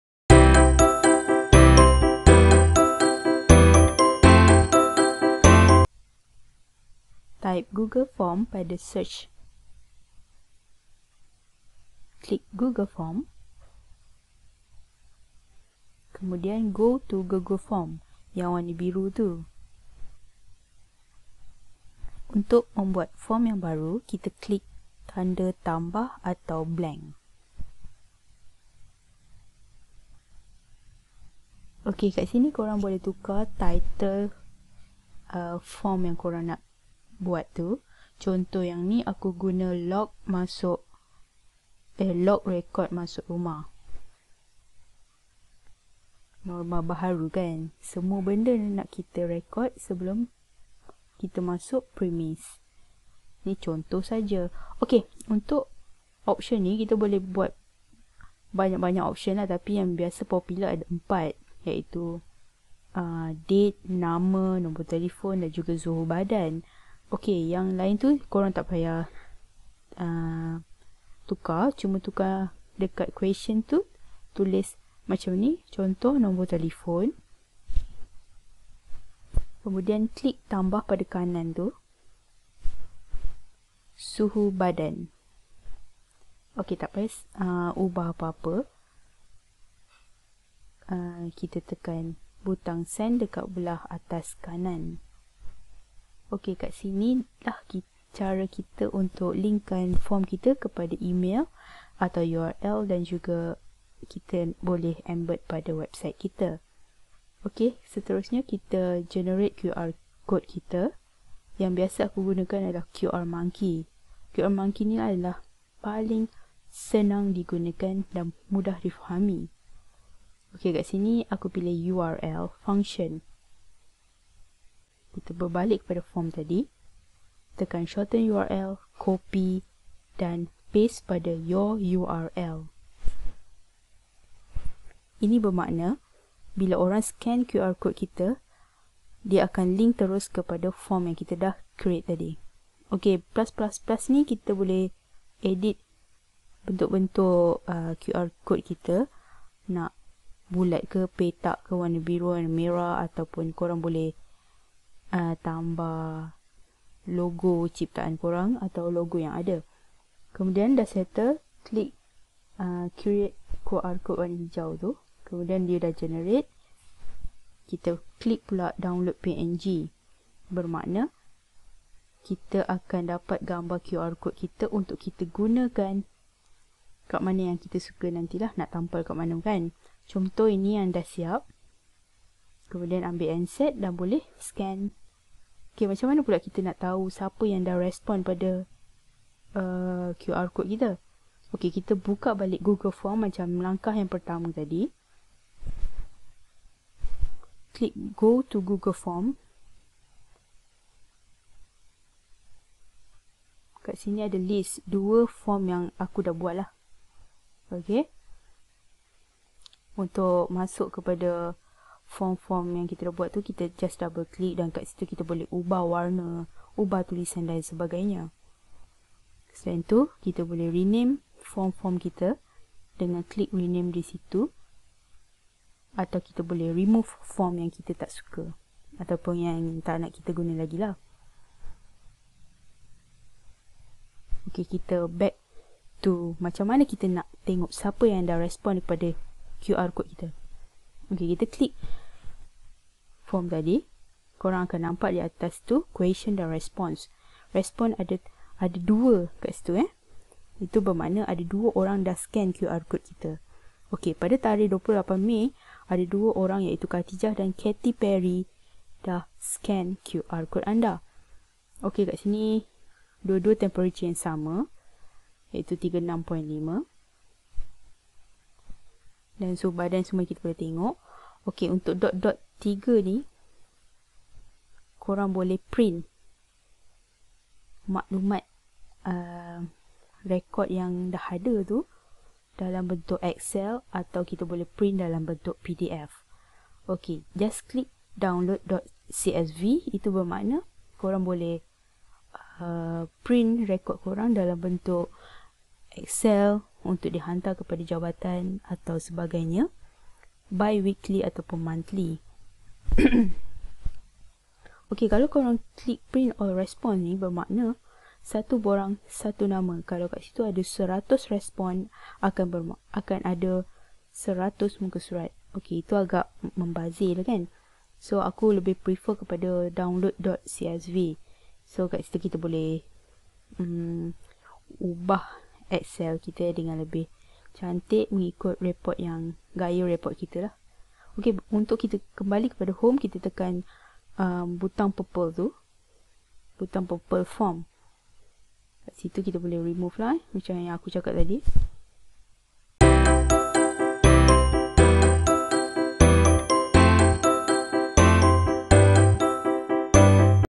TUTORIAL || CARA BUAT QR CODE SENDIRI DENGAN MUDAH
😍HANYA MENGGUNAKAN GOOGLE FORM DAN QR MONKEY SAHAJA😍